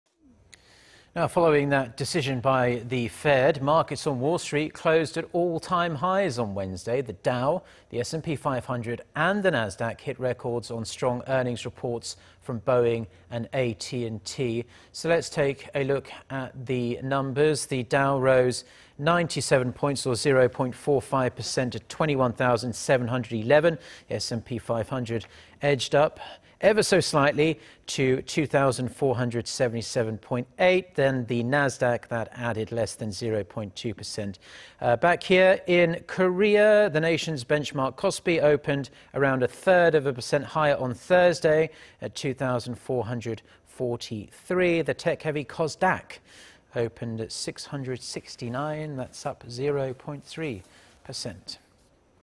stocks
Following the Fed's decision to hold rates,..
Markets on Wall Street closed at all-time highs on Wednesday-
The Dow, the S&P 500 and the Nasdaq hit records on strong earnings reports from Boeing and AT&T.
Looking at the numbers:
  The Dow rose 97 points, or zero-point-four-five percent, to 21,711, the S&P 500 edged up ever so slightly to 2,477-point-eight and the Nasdaq added less than zero-point-two percent to 6,422-point-seven.
  Back here in the country, Korea's benchmark KOSPI opened around a third-of-a-percent higher on Thursday at 2-thousand-four-hundred-43.
The tech heavy KOSDAQ opened at 6-hundred-69, up zero-point-three percent.